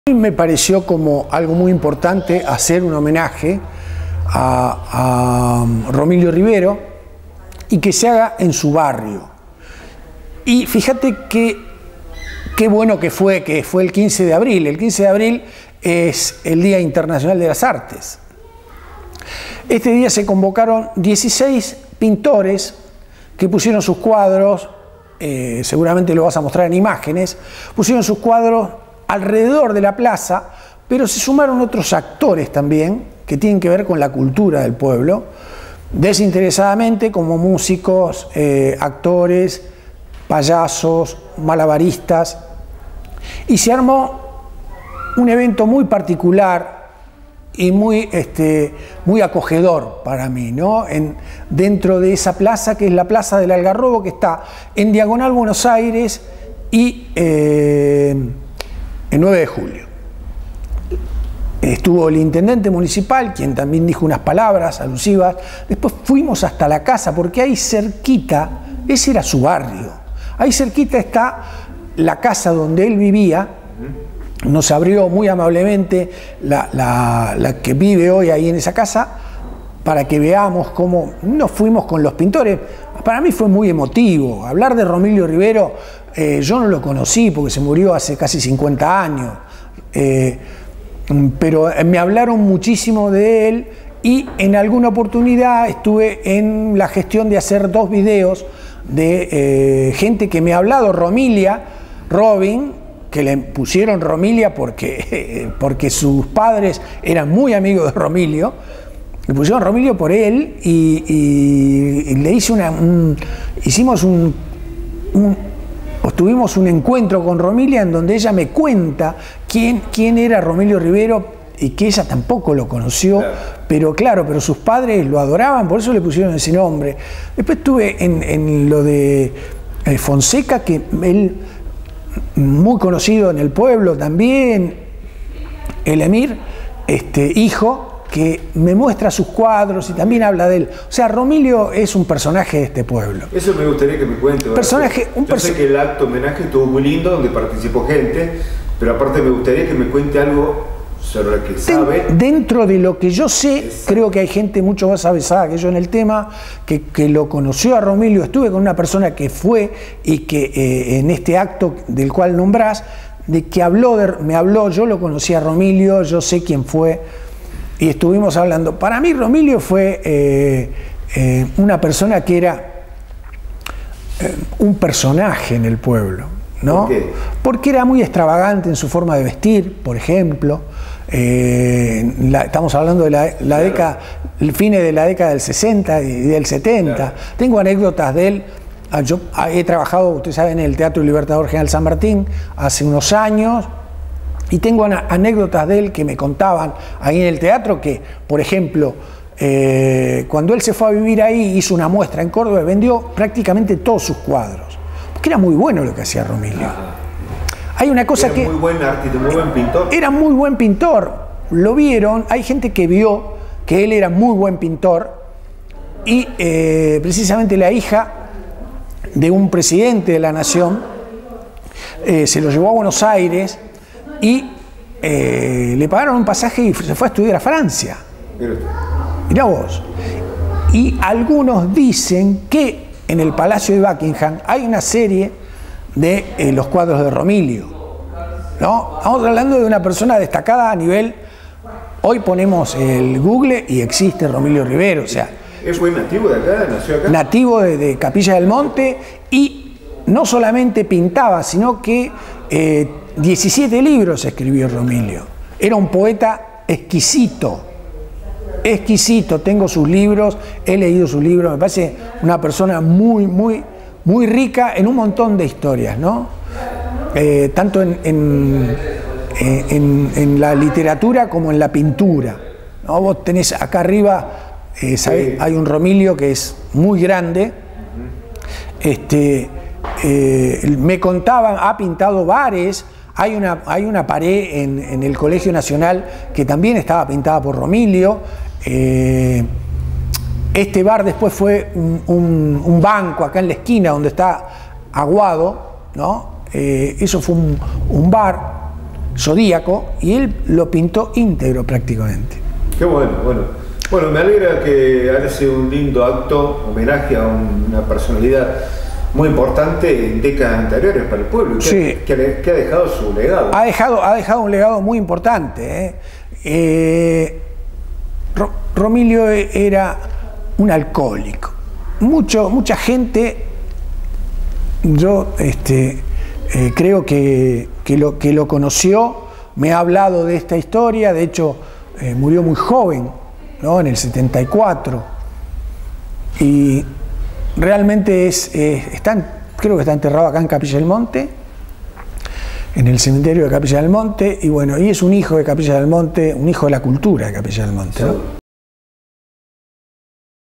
CAPILLA DEL MONTE HOMENAJE A ROMILIO RIBERO
Capilla del Monte, Abril 2023.
Romilio Ribero fue un escritor, poeta y pintor cordobés nacido en Capilla del Monte en el año 1933. Logró vivir de su arte y de sus vicios por 43 años. Entre la vida y el exceso, surgió su más vasta obra que incluye pinturas increíbles y poemas excepcionales. La municipalidad de Capilla del Monte le rindió un homenaje.